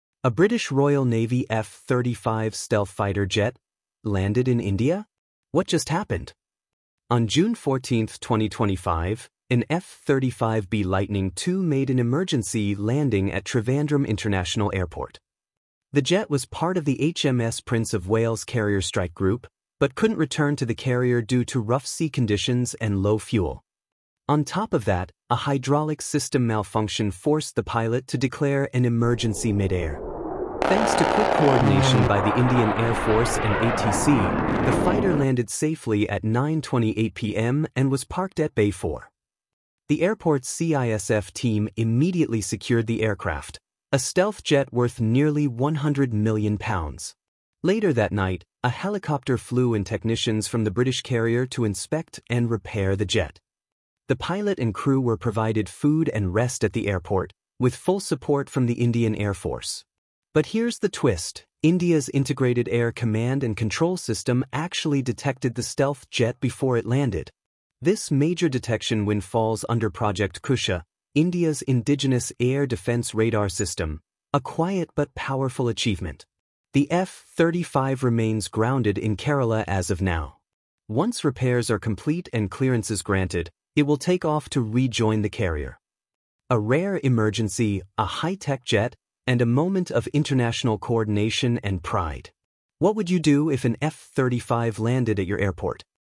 Why did a British f-35 stealth jet make an emergency landing in India?
A UK royal navy f-35b lightning ii stealth fighter jet made an emergency landing at Trivandrum airport, Kerala, on June 14, 2025. the jet, returning to the HMS Prince of wales aircraft carrier, faced a hydraulic snag and low fuel while operating in the Indian ocean. unable to land on the carrier due to rough seas, the pilot diverted and safely landed in India. the Indian air force coordinated the emergency landing and CISF secured the jet. technicians were flown in to repair it, while India's radar systems successfully detected the stealth aircraft — a rare feat. here's what happened, and what comes next.